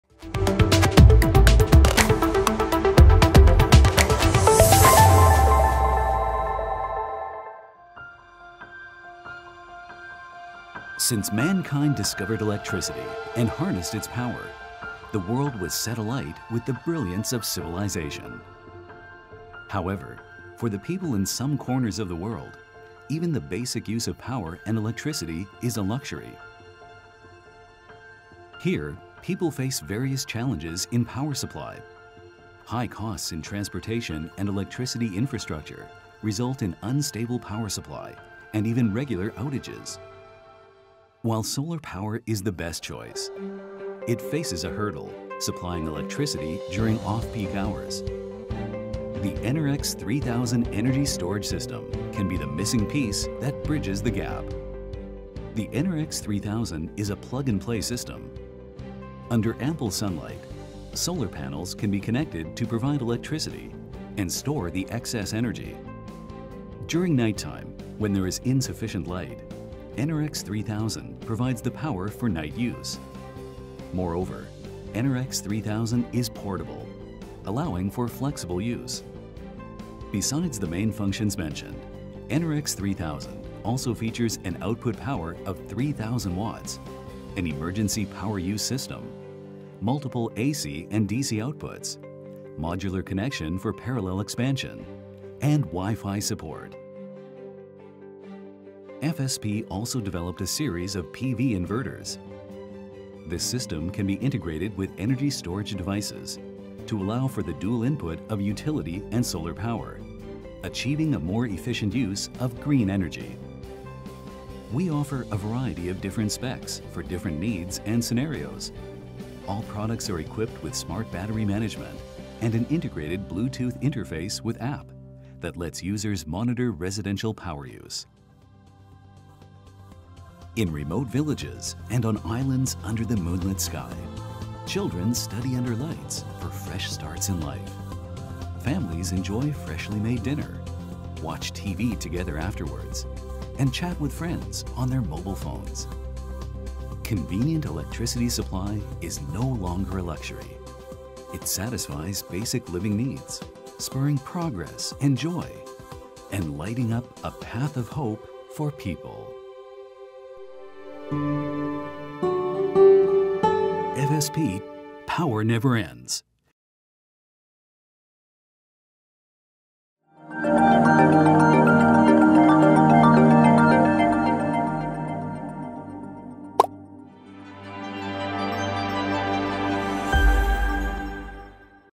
【Featured Product】Energy Storage System & PV Inverter Solutions
Some areas experience an unstable power supply and even regular outages. Unable to sustain basic living conditions, people seek out affordable and flexible sources of power. While solar power is the best choice, it faces a hurdle: supplying electricity during off-peak hours. The EnerX 3000 energy storage system developed by FSP can be the missing piece that bridges the gap. FSP also developed a series of PV inverters. This system can be integrated with energy storage devices to allow for the dual input of utility and solar power, achieving a more efficient use of green energy.